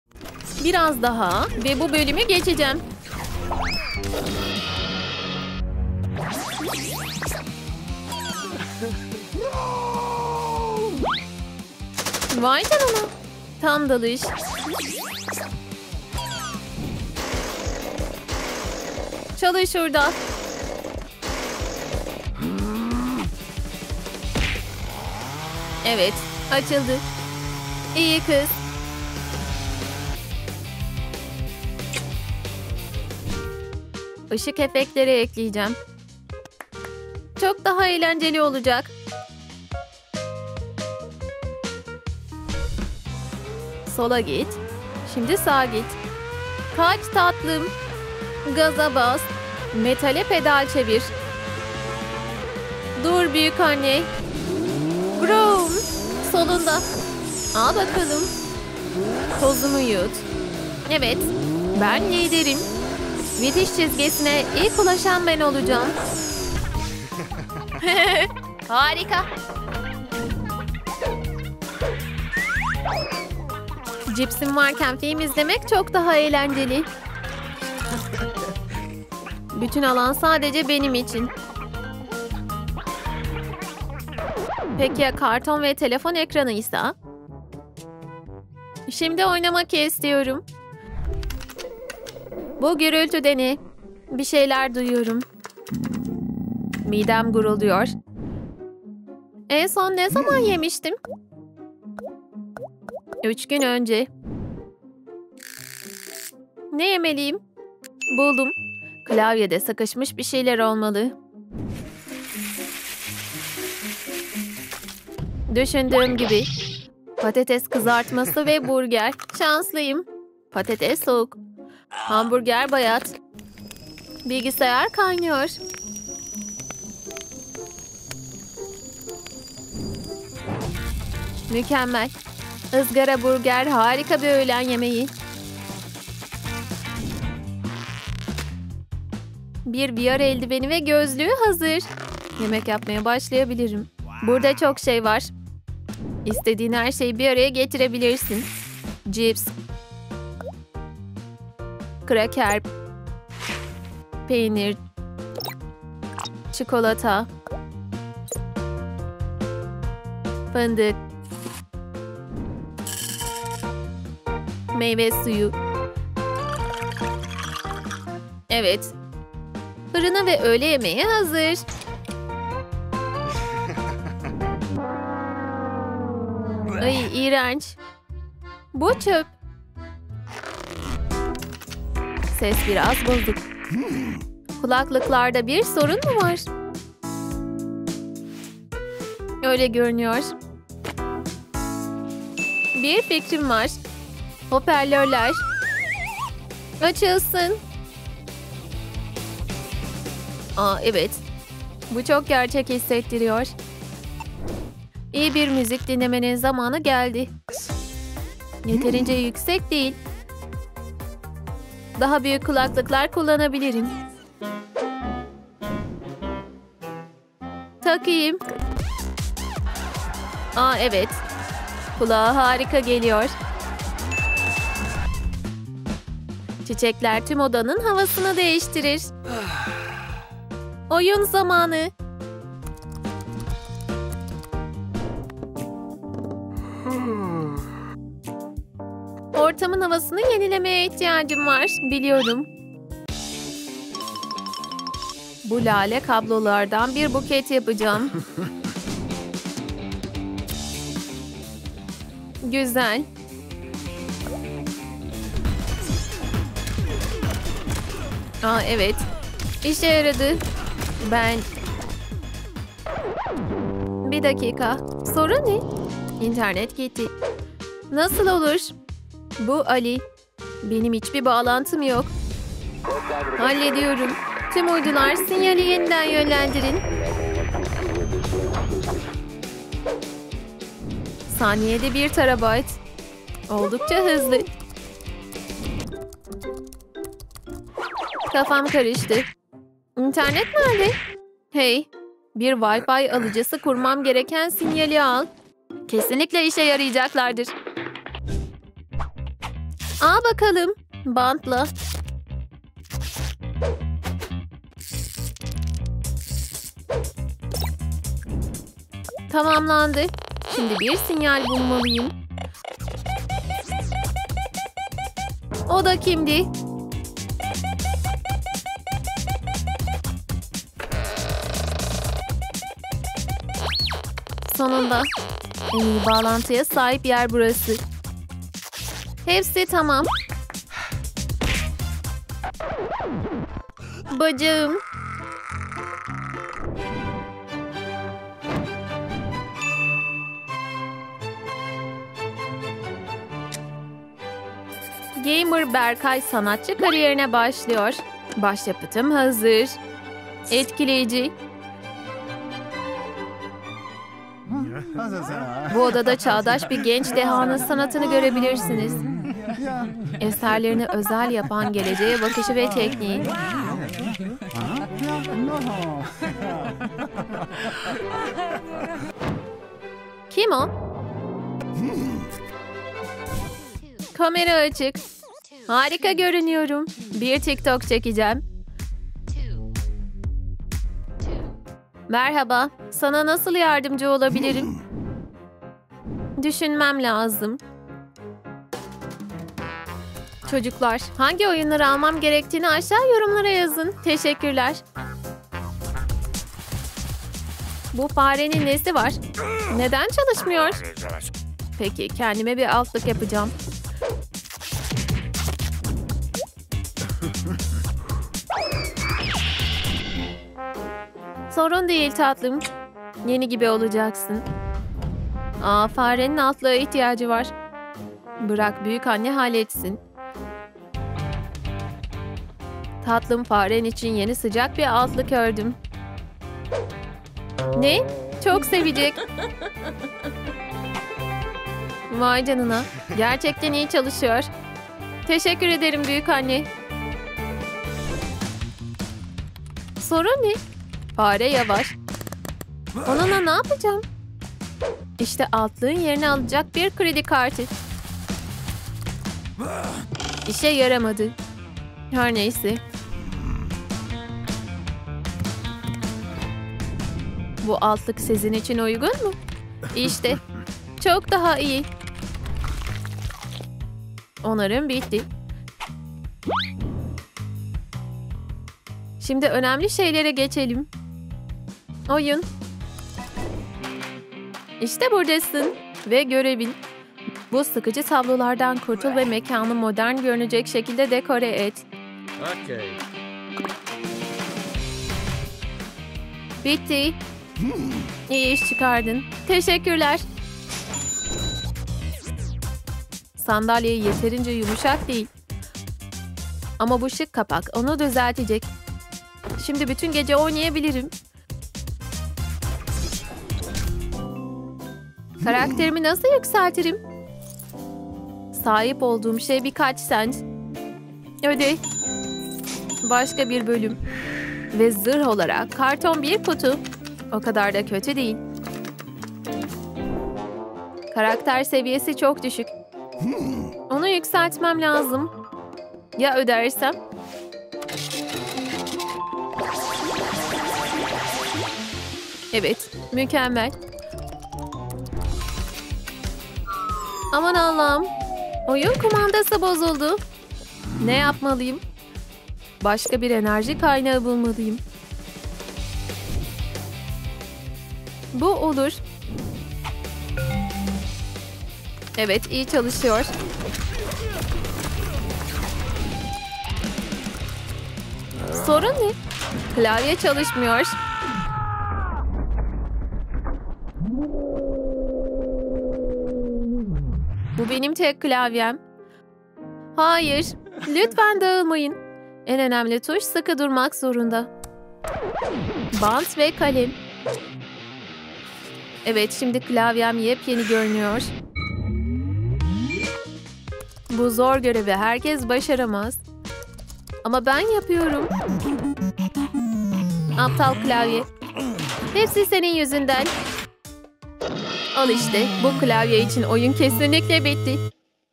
Zengin Gamer vs Fakir Gamer
Ali her şeyi ödeyebiliyor. Öte yandan Berkay ise oyunlarını geliştirmek için yeni yollar buluyor. Sizce kimin oyun oynaması daha ilginç?